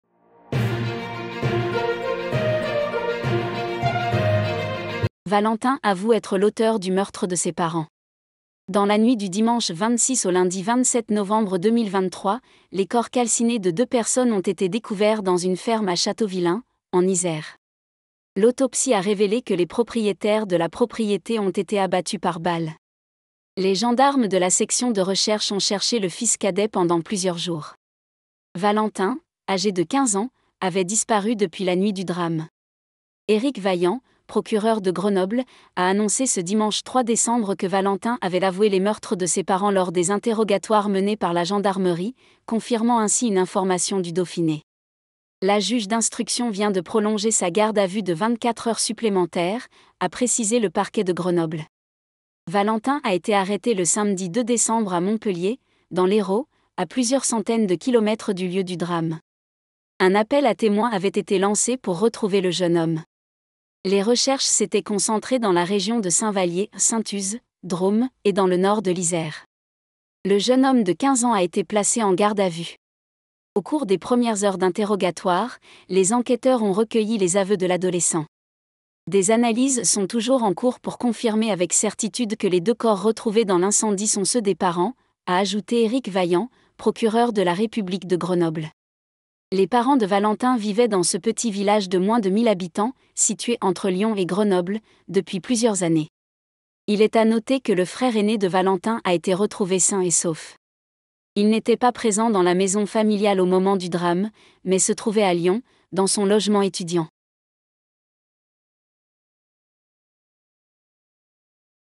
"Choc en Isère : Un adolescent de 15 ans avoue avoir tué ses parents"
"Adolescent de 15 ans avoue le meurtre de ses parents dans un incendie en Isère : Choc à Châteauvilain"